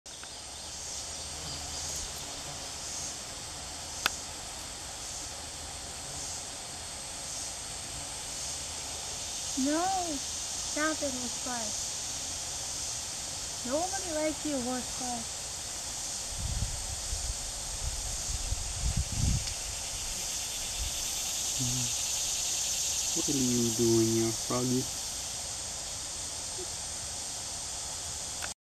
Floating Bullfrog
Bullfrog floating in the creek